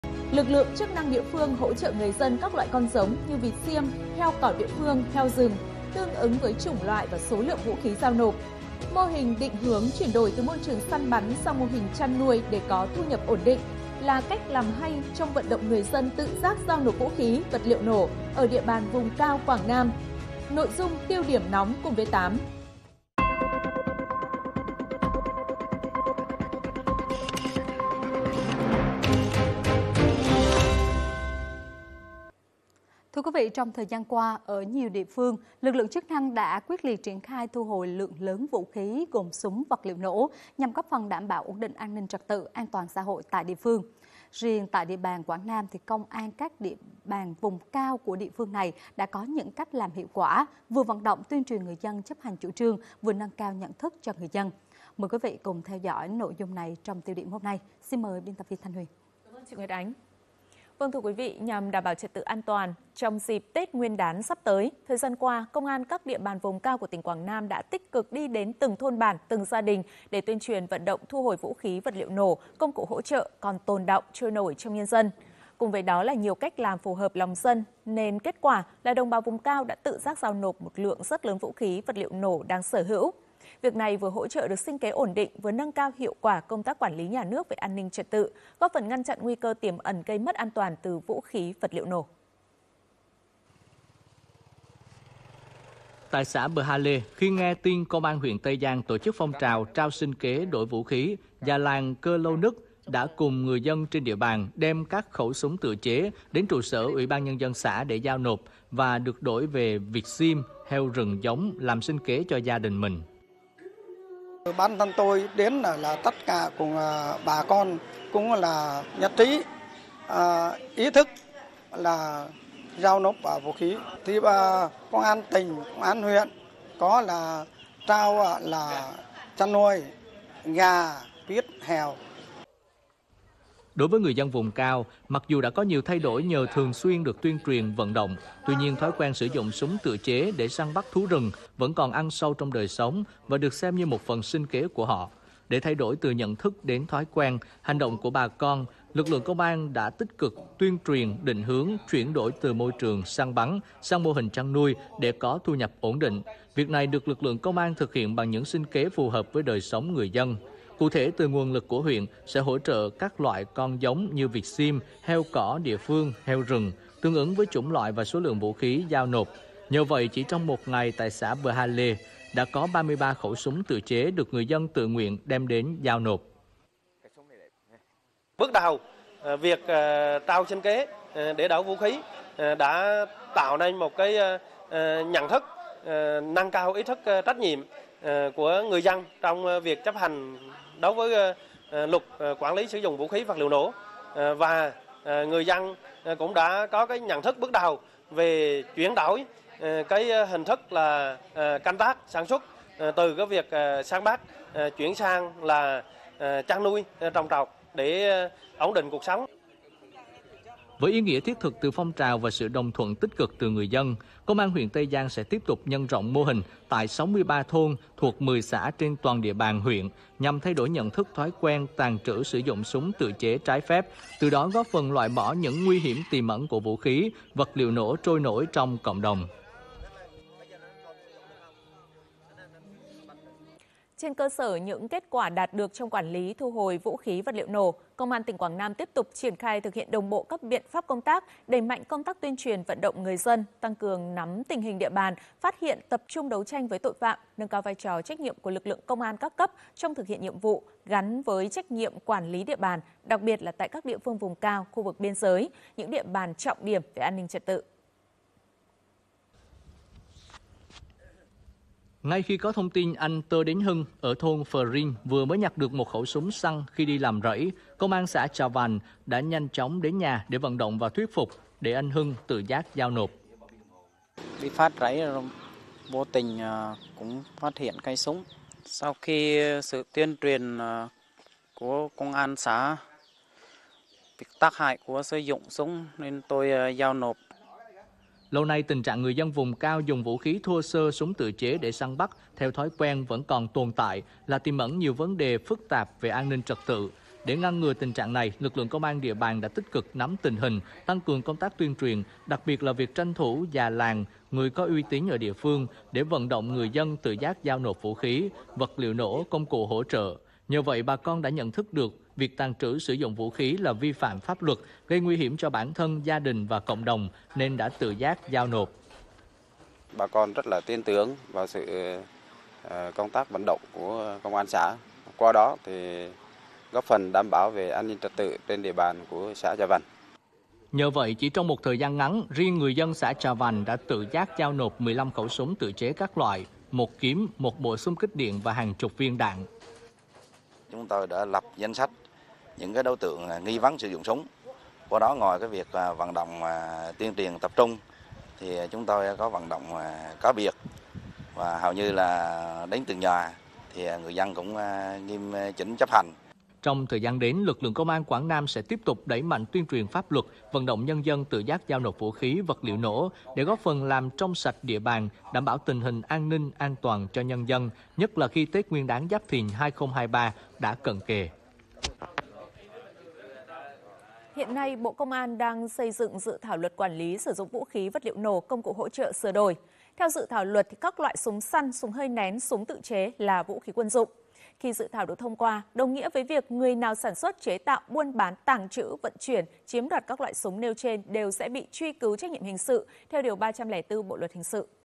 Đồng bào vùng cao Quảng Nam tự nguyện giao nộp vũ khí, vật liệu nổ
Nhằm đảm bảo trật tự, an toàn trong dịp Tết Nguyên đán sắp đến, thời gian qua, Công an các địa bàn vùng cao của tỉnh Quảng Nam đã tích cực đi đến từng thôn bản, từng gia đình để tuyên truyền, vận động thu hồi , vật liệu nổ, công cụ hỗ trợ còn tồn đọng, trôi nổi trong nhân dân.